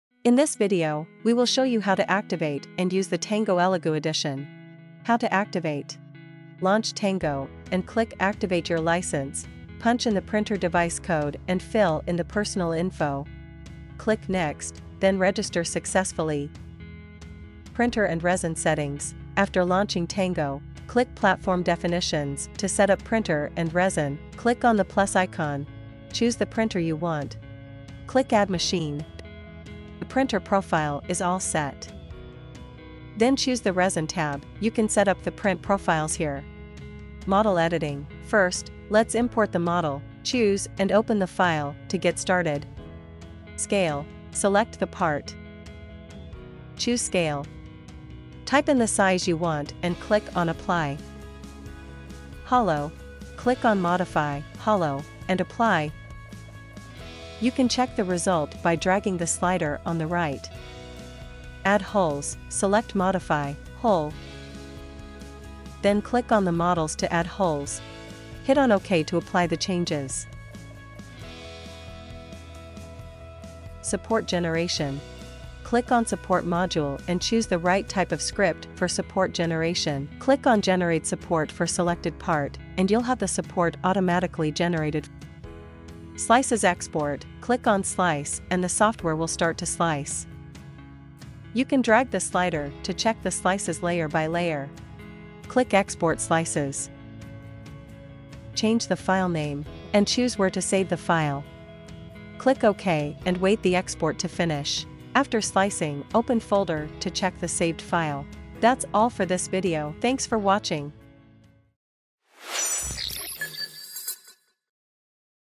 Beginners' Guide to Tango Elegoo Edition | Voxeldance Tango
In this video, we will show you how to activate and use the Tango Elegoo Edition, which includes steps from activating Tango Elegoo Edition with the Elegoo printer code to slicing your first model!

00:00 - Intro
00:05 - How to activate
00:17 - Printer and resin settings
00:37 - Model editing
00:43 - Scale
00:51 - Hollow
01:02 - Add holes
01:14 - Support generation
01:26 - Slices export
01:52 - Outro

Model creator: Ernest Nemirovskiy